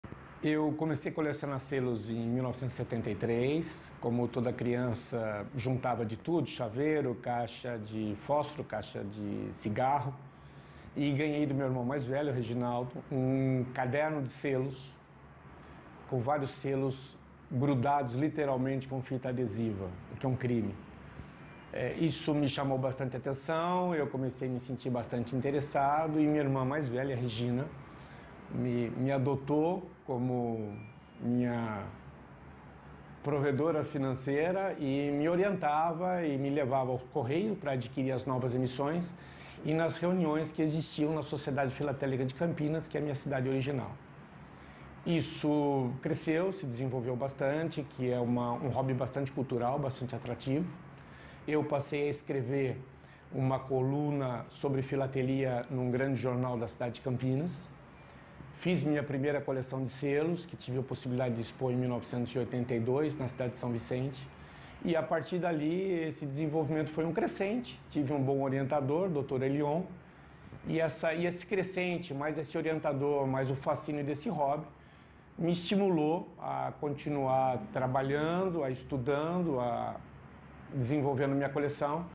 Filatelista conta como começou sua coleção de selos
ESPECIAL WEB - 31.07.15: Primeiro de agosto é o Dia do Selo Postal Brasileiro. O Presidente da Federação Brasileira de Filatelia (Febraf), Reinaldo Macedo, conta como desenvolveu sua coleção, ainda na infância.